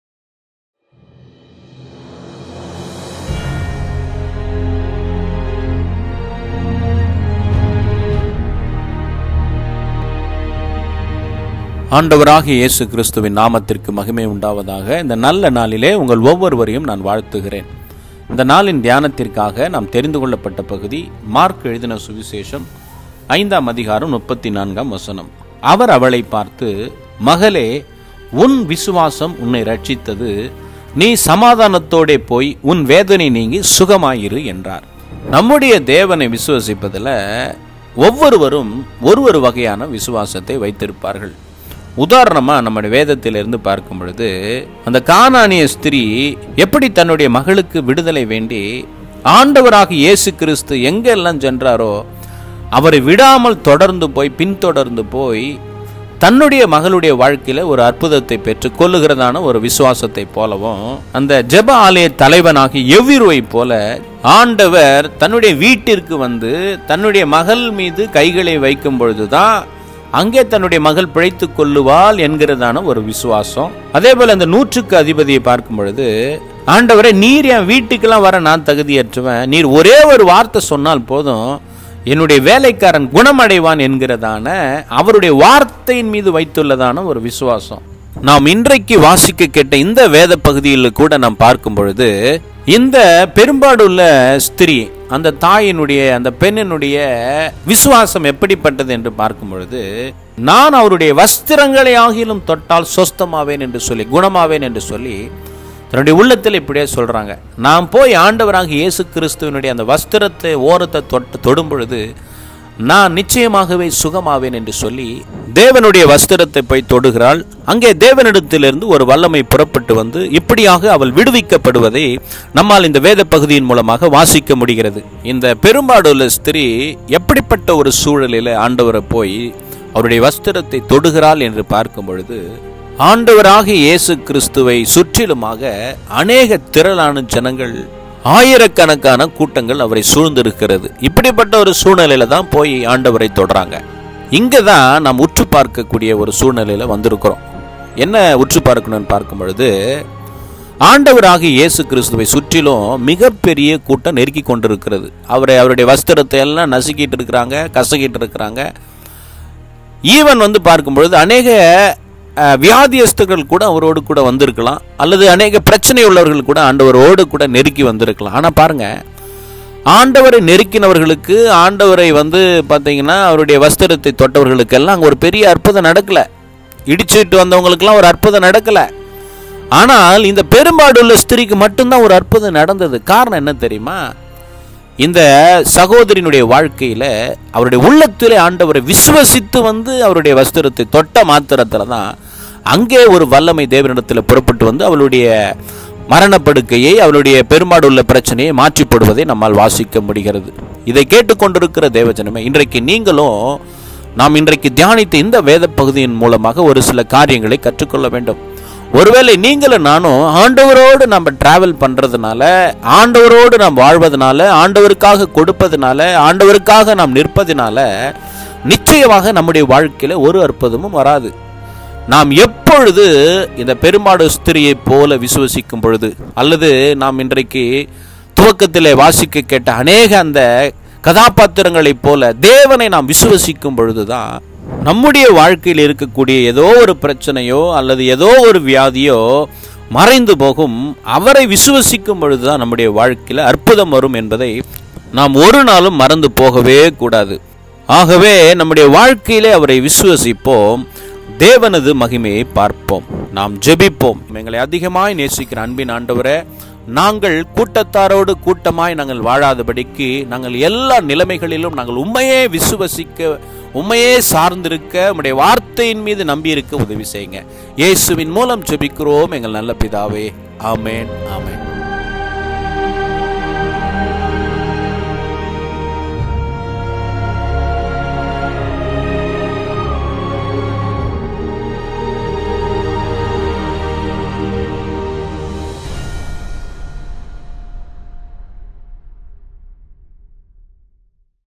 Daily strength 07.08.2020
Let us seek the reason for losing faith and the way to restore our faith together.